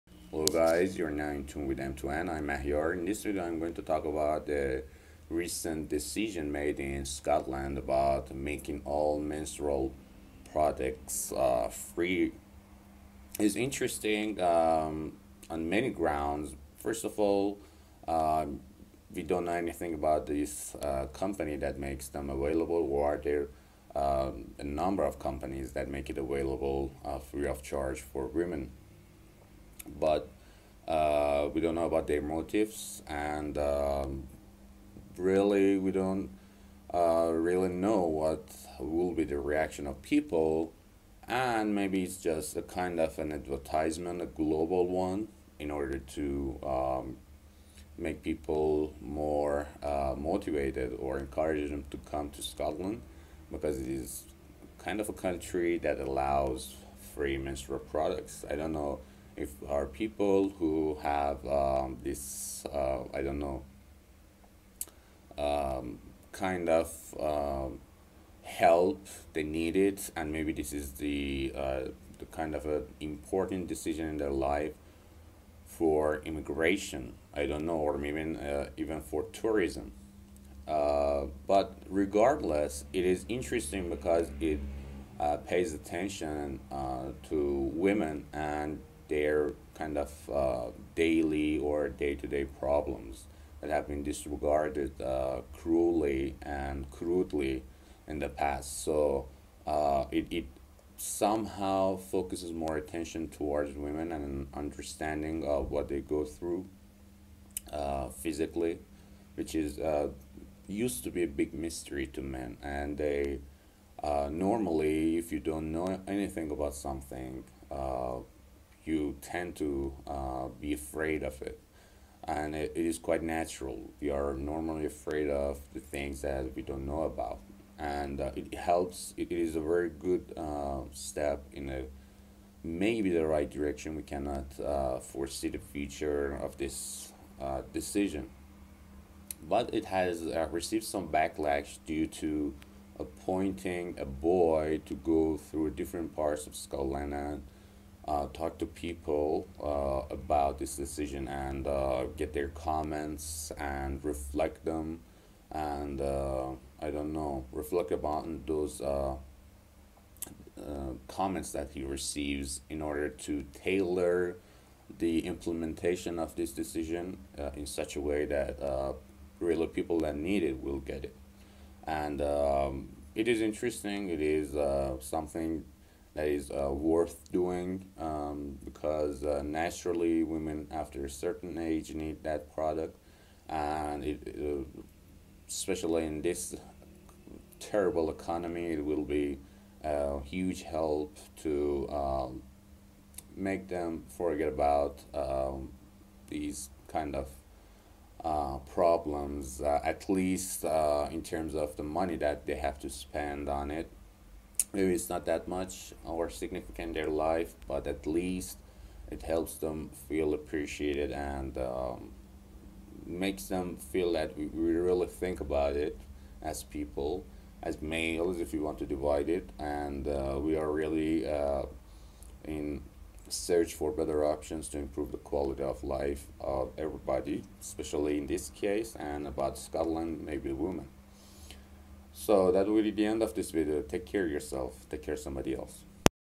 Free women products in Scotland!
Recently Scotland has made menstural products free , here’s my take on it.